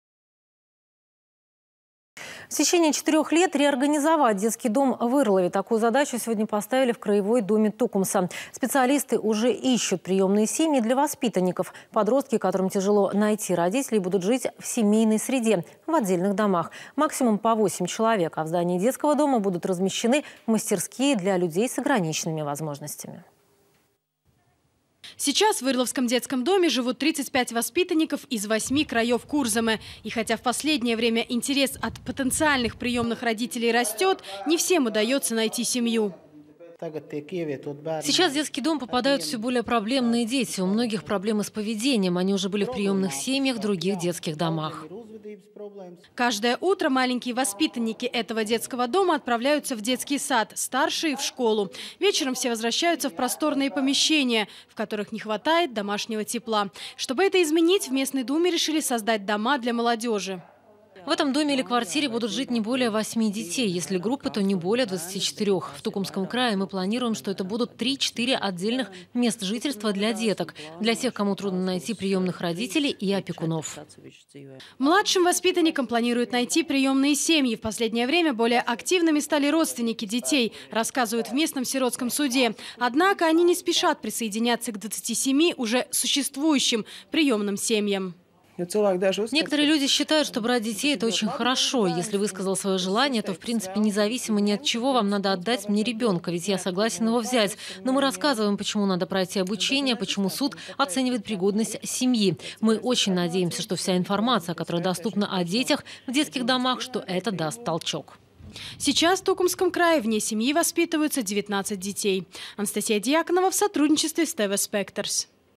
Ирлавский детский дом будет реорганизован
В течение четырёх лет реорганизовать детский дом в Ирлаве. Такую задачу сегодня поставили в краевой думе Тукумса. Специалисты уже ищут приёмные семьи для воспитанников. Подростки, которым тяжело найти родителей, будут жить в семейной среде в отдельных домах. Максимум — по 8 человек. А в здании детского дома будут размещены мастерские для людей с ограниченными возможностями.